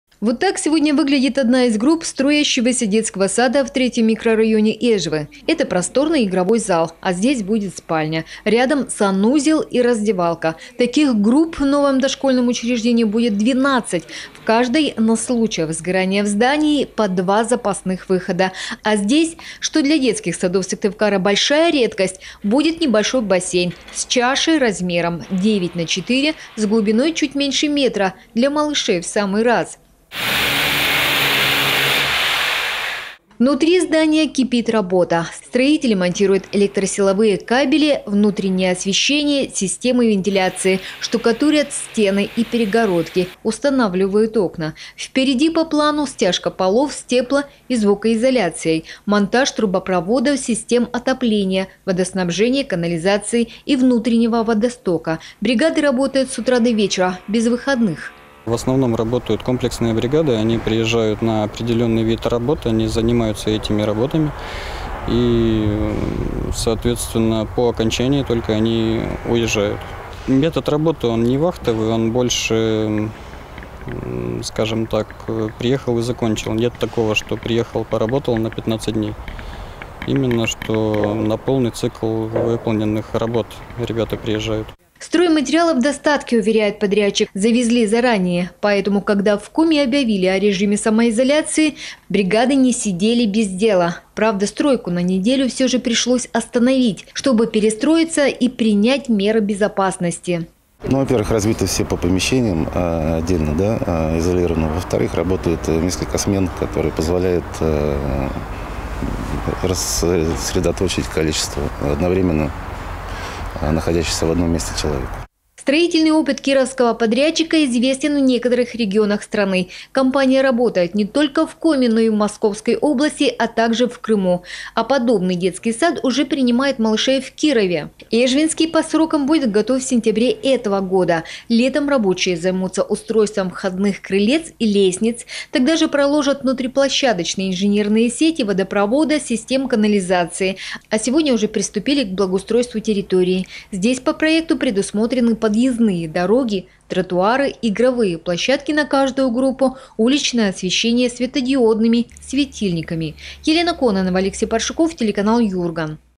Эжвинский детский сад будет построен в установленные сроки
Эжвинский детский сад на 270 мест подрядчик готов сдать  в установленные сроки - то есть до сентября этого года. На сегодня здесь завершаются кровельные работы, проходит внутренняя отделка помещений, монтаж освещения и оконных блоков. Строительство дошкольного учреждения ведется в рамках реализации нацпроекта «Демография».